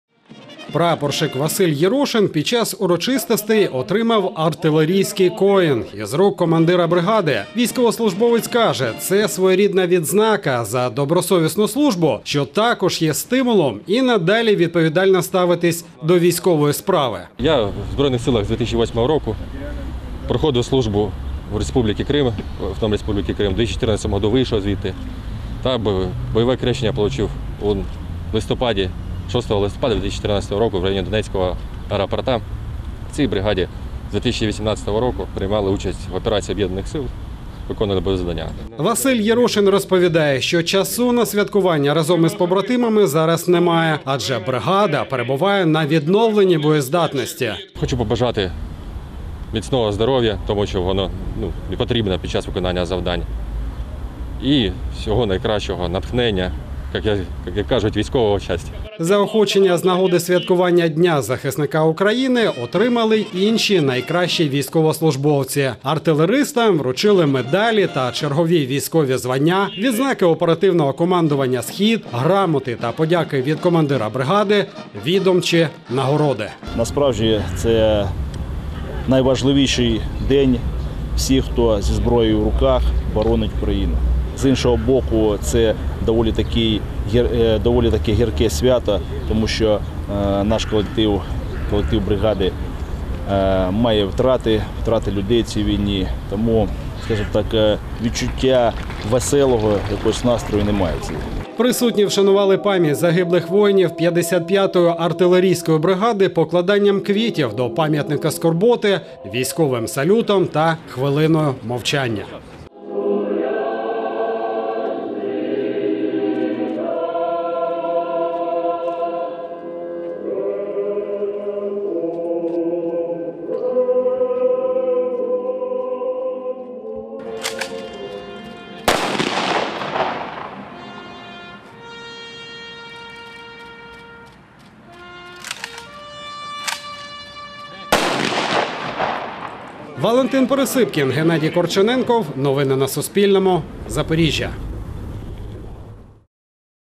День захисника України|Новини|12.10.2020
У запорізькій 55-й окремій артилерійській бригаді «Запорізька Січ» відбулись урочистості з нагоди Дня захисника України, що відзначатиметься 14 жовтня. Засвідчити повагу тим, хто у різні часи боронив Батьківщину та відстоював територіальну цілісність і незалежність країни, зібралися керівництво та особовий склад бригади, а також запрошені гості.